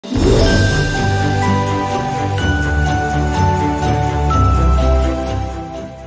SEASONAL RELOAD BONUS WITH HUTCH!
25% RELOAD BONUS THIS FESTIVE SEASON WITH HUTCH! Recharge any of the mentioned reload amounts above from the HUTCH APP, any DIGITAL BANKING APP or the nearest supermarket between the 23rd of December 2023 and 3rd of January 2024! *Prepaid Only *T&C Apply